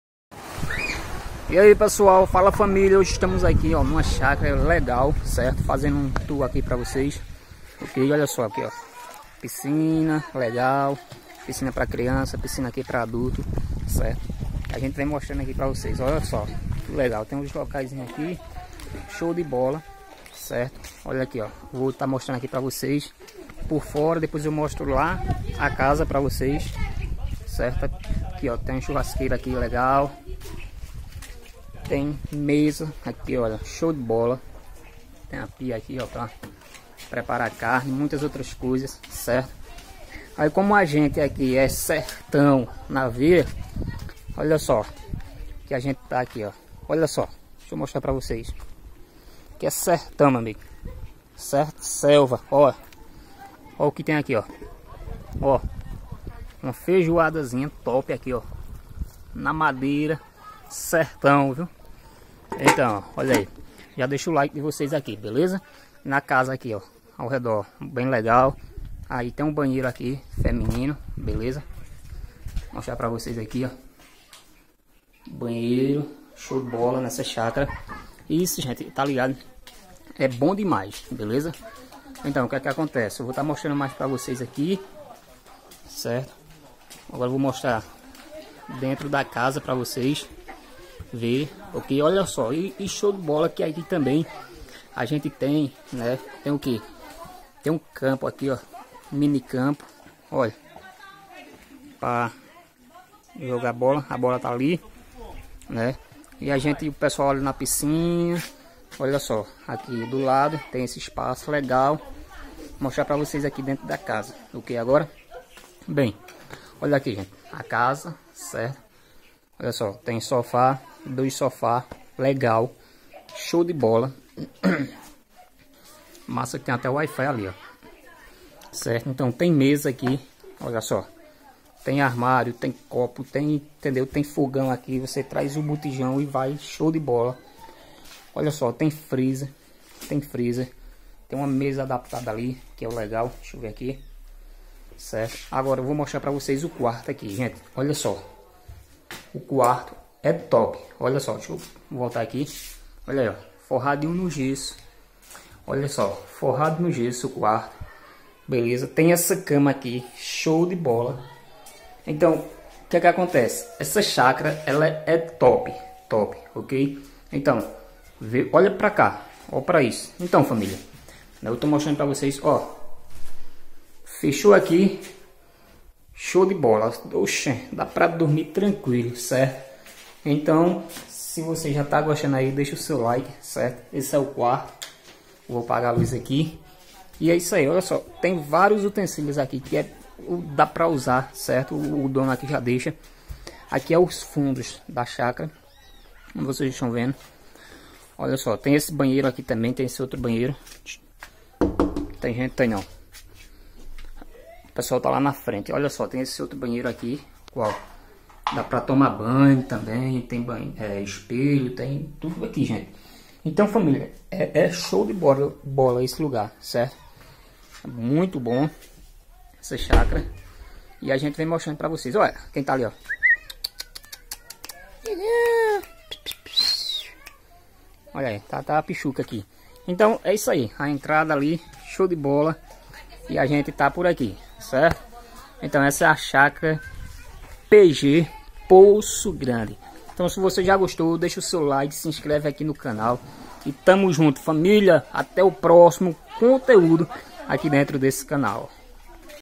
um dia de lazer com a familia na chácara
hoje fiz um tour pela casa da chácara vocês poodem acompanhar